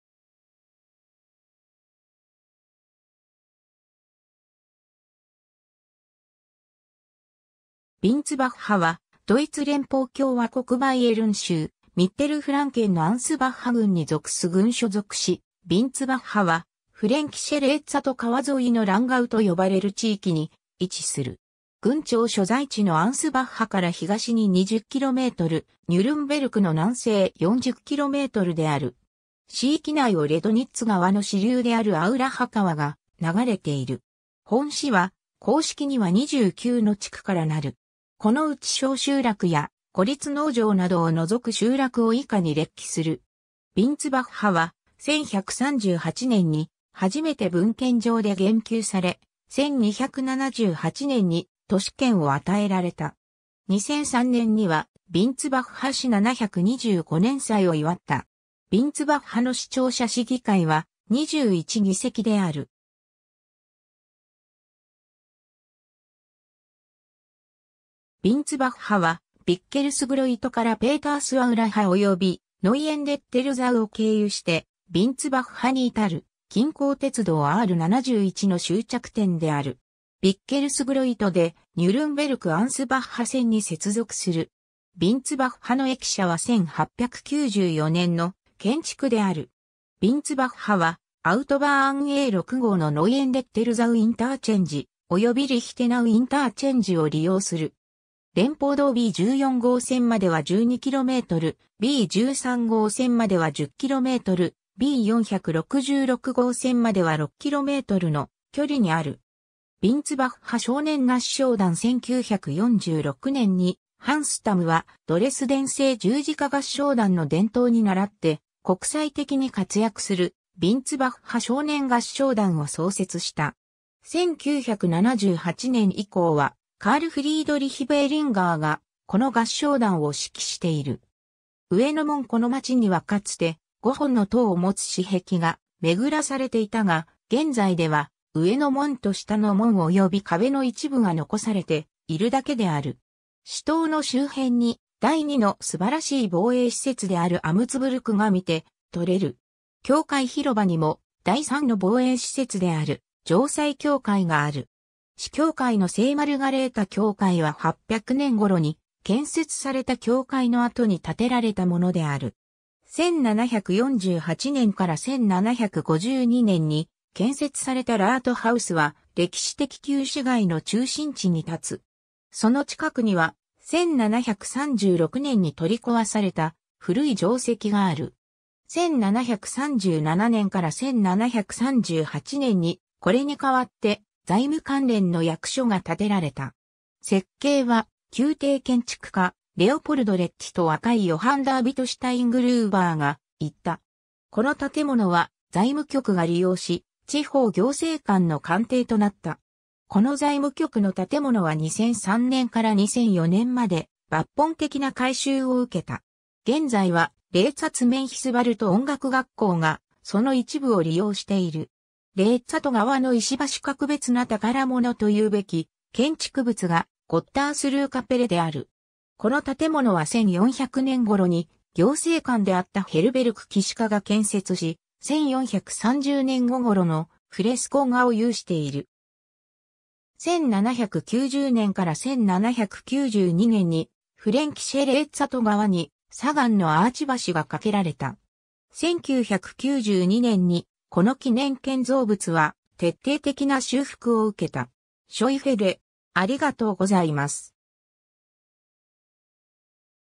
ヴィンツバッハ
ヴィンツバッハ (ドイツ語: Windsbach) は、ドイツ連邦共和国バイエルン州ミッテルフランケンのアンスバッハ郡に属す郡所属市 ヴィンツバッハはフレンキシェ・レーツァト川沿いのランガウと呼ばれる地域に位置する。
郡庁所在地のアンスバッハから東に20km、ニュルンベルクの南西40kmである。
市域内をレドニッツ川の支流であるアウラハ川が流れている。
 本市は、公式には29の地区 (Ort) からなる。
 ヴィンツバッハは、1138年に初めて文献上で言及され、1278年に都市権を与えられた。
2003年にはヴィンツバッハ市725年祭を祝った。
 ヴィンツバッハの市庁舎 市議会は、21議席（第1市長を含む）である。
 ヴィンツバッハは、ヴィッケルスグロイトからペータースアウラハおよびノイエンデッテルザウを経由してヴィンツバッハに至る近郊鉄道R71の終着点である。
ヴィッケルスグロイトで、ニュルンベルク - アンスバッハ線(R7)に接続する。
ヴィンツバッハの駅舎は1894年の建築である。
 ヴィンツバッハは、アウトバーンA6(E50)号のノイエンデッテルザウ・インターチェンジおよびリヒテナウ・インターチェンジを利用する。
連邦道B14号線までは12km、B13号線までは10km、B466号線までは6kmの距離にある。
 ヴィンツバッハ少年合唱団 1946年にハンス・タムは、ドレスデン聖十字架合唱団の伝統にならって、国際的に活躍するヴィンツバッハ少年合唱団を創設した。
1978年以降はカール＝フリードリヒ・ベーリンガーがこの合唱団を指揮している。
 上の門 この街にはかつて、5本の塔を持つ市壁が巡らされていたが、現在では上の門（シュヴァーバッハ門）と下の門（ブリュッケン門）および壁の一部が遺されているだけである。
市塔（12世紀）の周辺に第2のすばらしい防衛施設であるアムツブルクが見て取れる。
教会広場にも第3の防衛施設である城塞教会がある。
市教会の聖マルガレータ教会（1730年建設）は800年頃に建設された教会の跡に建てられたものである。
 1748年から1752年に建設されたラートハウス（市庁舎）は、歴史的旧市街の中心地に建つ。
その近くには、1736年に取り壊された古い城跡がある。
1737年から1738年に、これに変わって財務関連の役所が建てられた。
設計は宮廷建築家レオポルド・レッティと若いヨハン・ダーヴィト・シュタイングルーバーが行った。
この建物は、財務局が利用し、地方行政官の官邸となった。
この財務局の建物は2003年から2004年まで抜本的な改修を受けた。
現在は、レーツァツ＝メンヒスヴァルト音楽学校がその一部を利用している。
 レーツァト川の石橋 格別な宝物と言うべき建築物が、ゴッタースルーカペレ（神の安息礼拝堂）である。
この建物は1400年頃に行政官であったヘルベルク騎士家が建設し、1430年後頃のフレスコ画を有している。
 1790年から1792年に、フレンキシェ・レーツァト川に砂岩のアーチ橋が架けられた。
1992年にこの記念建造物は徹底的な修復を受けた。
 ショイフェレ （これらの文献はドイツ語版の参考文献として挙げられていたものであり、日本語版作成に際し直接参照してはおりません）